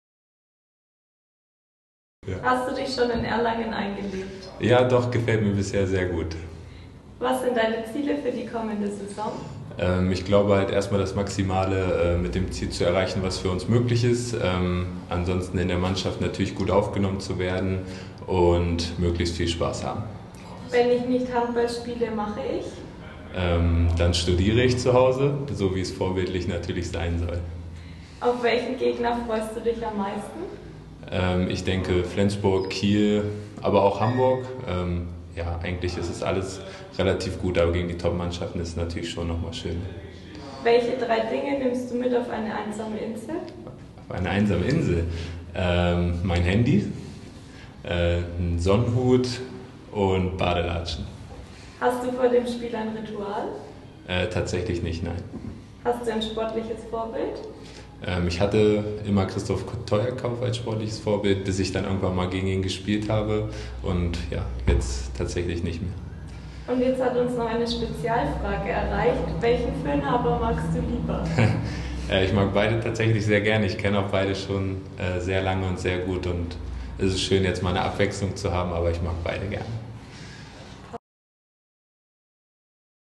🔵🔴 8 Fragen an Tim Zechel
Unser Neuzugang mit der Nummer 96 - Tim Zechel beantwortet Euch 8 Fragen. Viel Spaß!